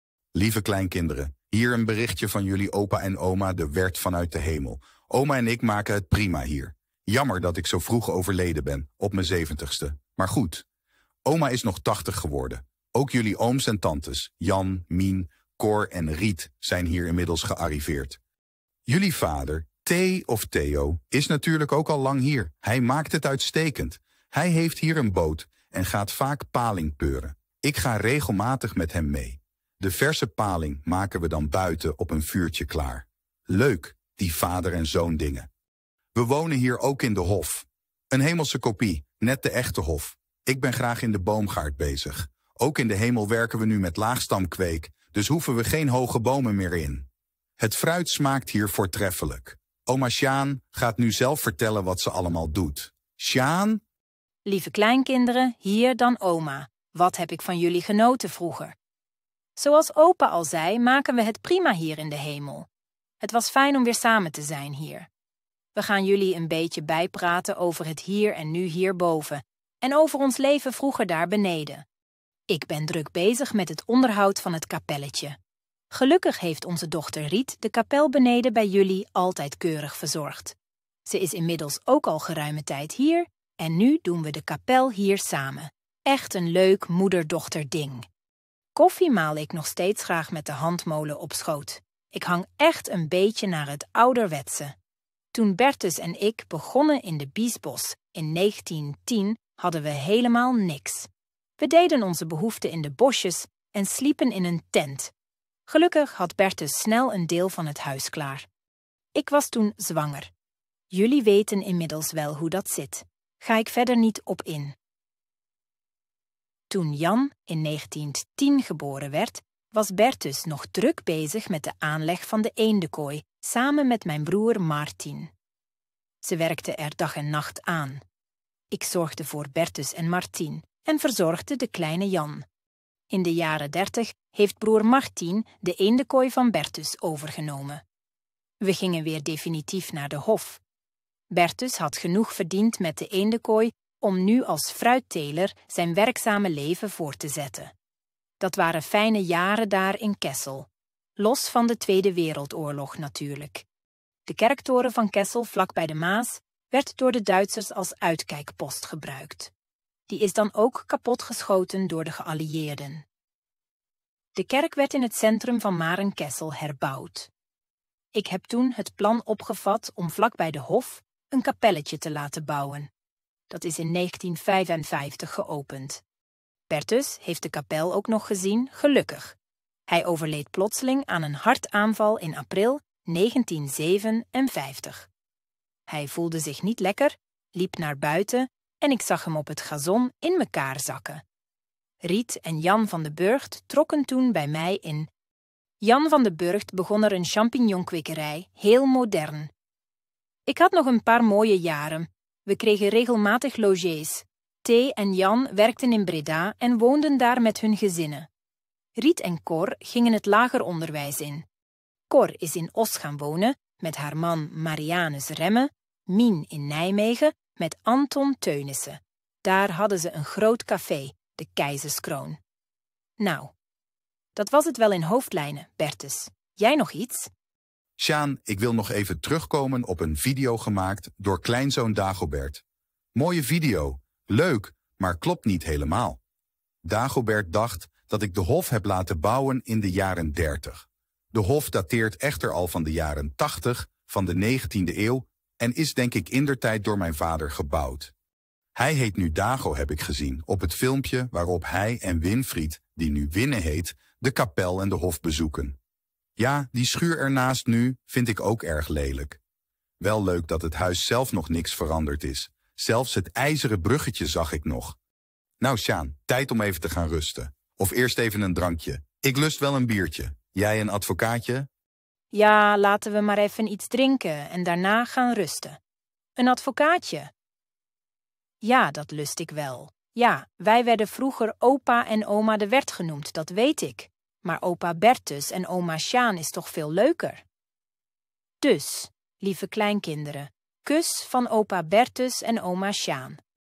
Opa en oma de Werd spreken vanuit de hemel
Deze video is een geintje, gecreeerd met de online AI software van Hedra.